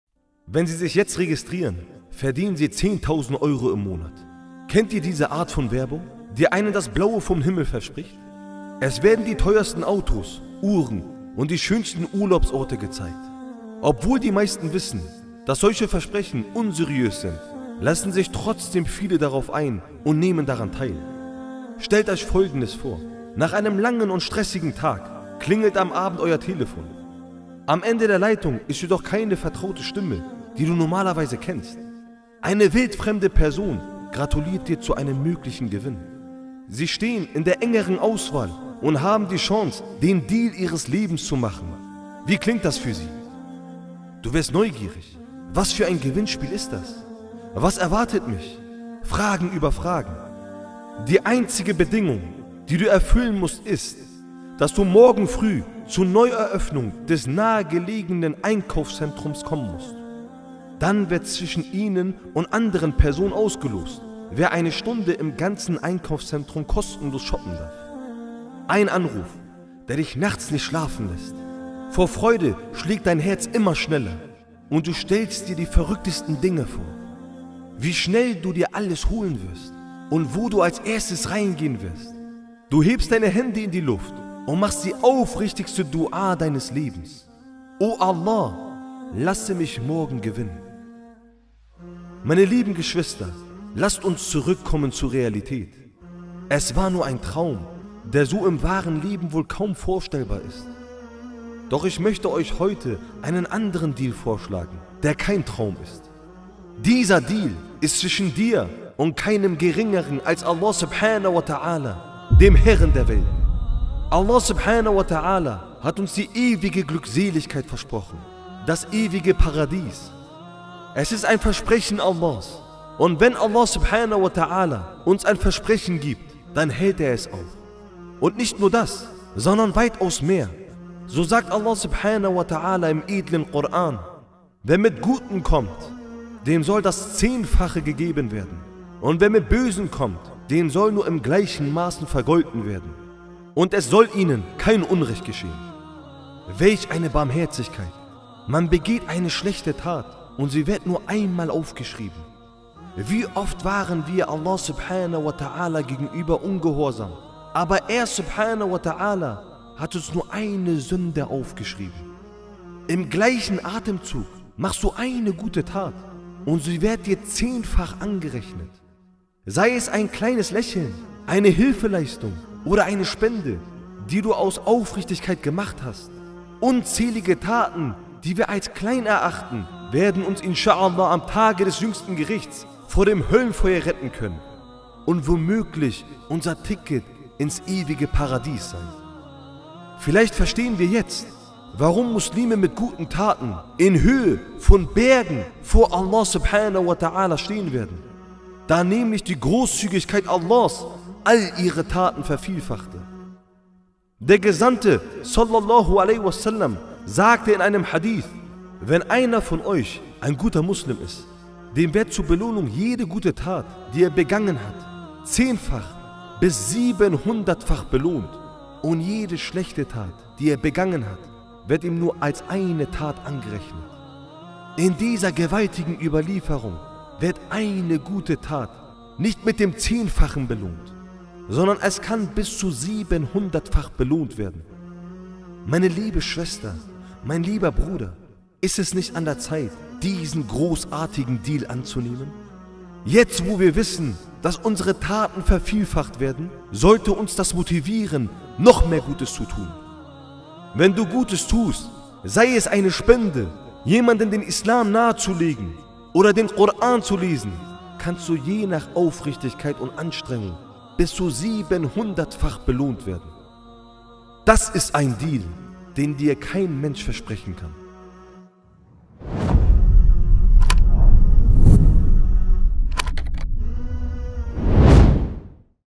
Verpasse niemals diesen Deal! ᴴᴰ ┇ Worte zum Nachdenken┇ BDI
Der Gesandte (sas) sagt in einem Hadith: „Wenn einer von euch ein guter Muslim ist, dem wird zur Belohnung jede gute Tat, die er begangen hat, zehnfach bis siebenhundertfach belohnt und jede schlechte Tat, die er begangen hat, wird ihm nur als eine Tat angerechnet.“ (Bukhari)